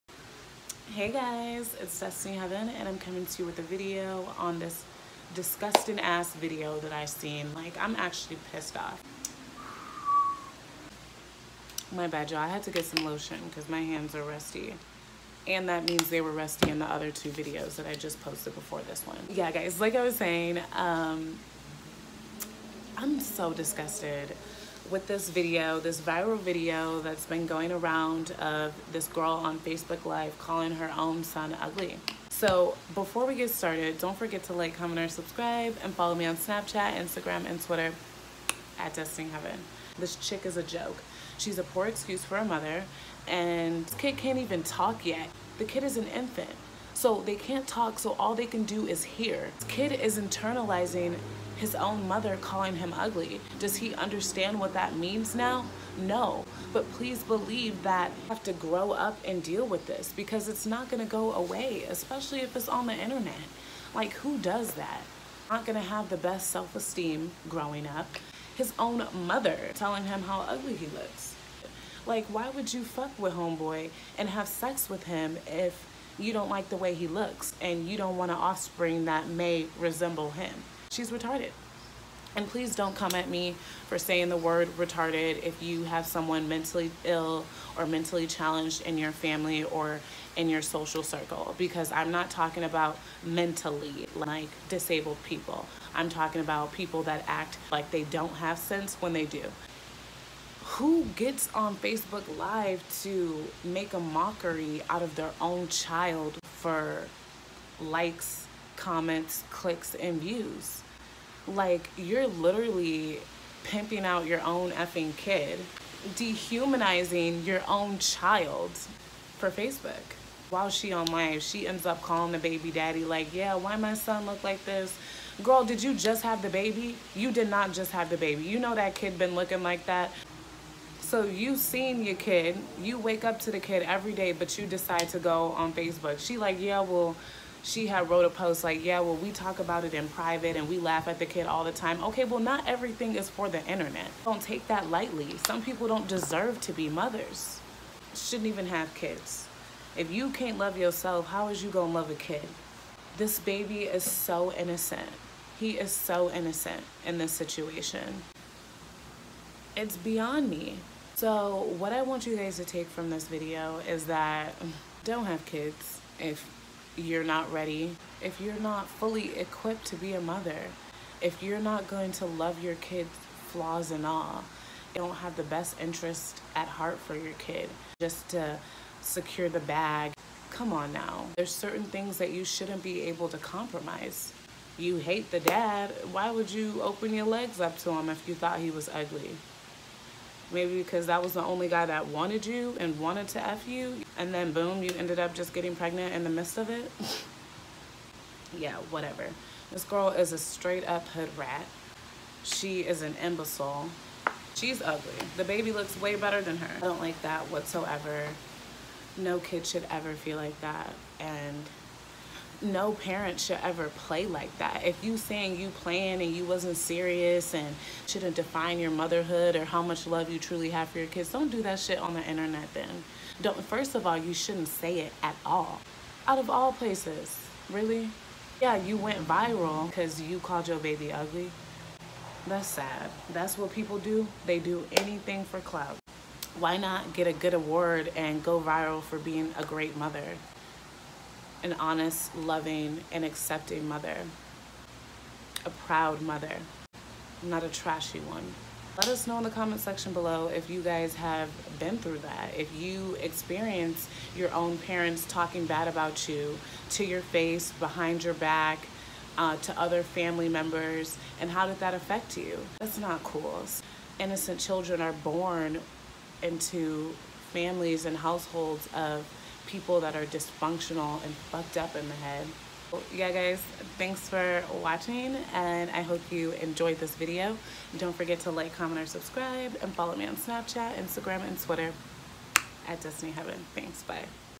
GIRL GOES VIRAL FOR CALLING HER BABY UGLY 😓😒😱
Hey guys💋. I came across the video of this girl calling HER OWN BABY UGLY😢 let me know how you feel about my commentary in comment section below! 🌸💕